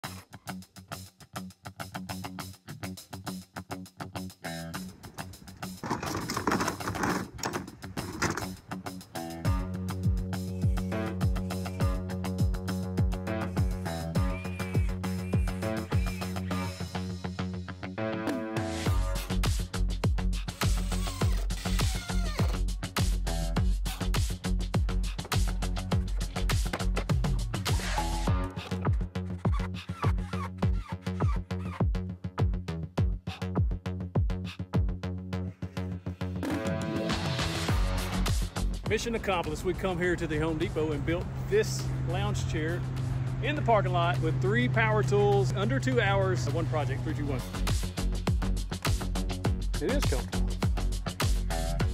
Using 3 Tools with less than 2 Hours to Build 1 Project
I am going to attempt to make 1 beginner woodworking project using only 3 power tools in less than 2 hours. Can I do it? Let's find out! A full build video is coming soon.